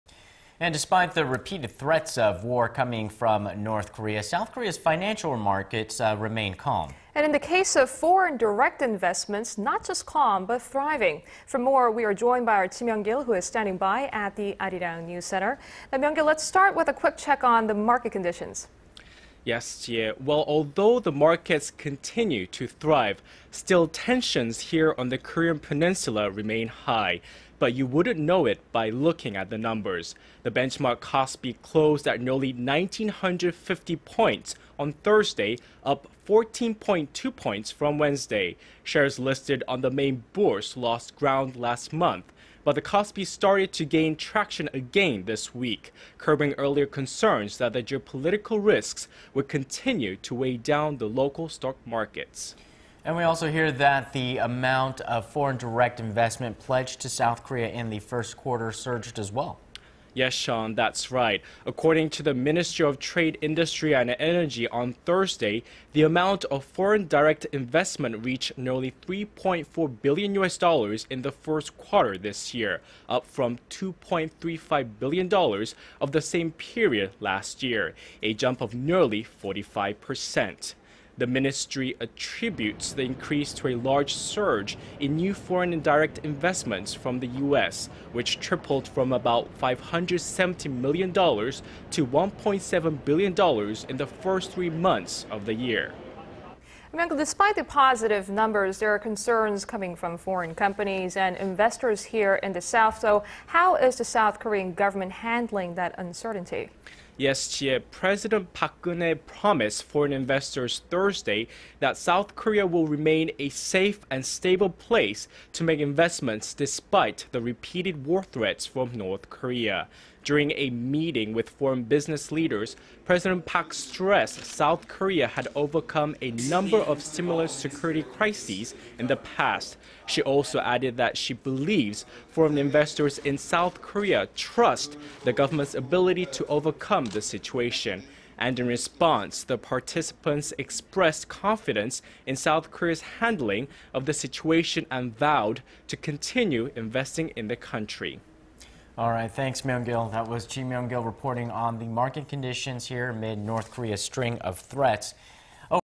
N.Korean threats have little impact on S.Korean financial markets 북한 위협에 한국증시 영향력 없어
Despite the repeated threats of war eminating out of North Korea, South Korea's financial markets remain calm.

And in the case of foreign direct investment, not just calm but thriving.

Here's Arirang News' Ji Myung-kil. North Korea's decision to pull its workers from the inter-Korean Gaeseong Industrial Complex and its missile and nuclear threats seem to have had little impact on Korea's financial markets.

Although tensions on the peninsula remain high, the benchmark KOSPI closed at nearly 19-hundred-50 points on Thursday, UP14-point-2 points from Wednesday.

Shares listed on the main bourse lost ground last month, but the KOSPI has started to gain traction again this week, curbing earlier concerns that the geopolitical risks would continue to weigh down the local stock markets.

According to the Ministry of Trade, Industry and Energy on Thursday, the amount of foreign direct investment reached 3-point-39 billion U.S. dollars in the first quarter of this year UP nearly 45 percent compared to the same period last year.

The ministry attributes the increase to a large surge in new foreign direct investments from the U.S., which tripled from about 567 million dollars to 1-point-7 billion dollars in the first three months of the year.

The increased presence from overseas investors in South Korea's secondary bourse has pushed it to levels seen before the 2008 financial crisis.

Market watchers say the buying spree came as foreign investors scooped up undervalued shares on the secondary market.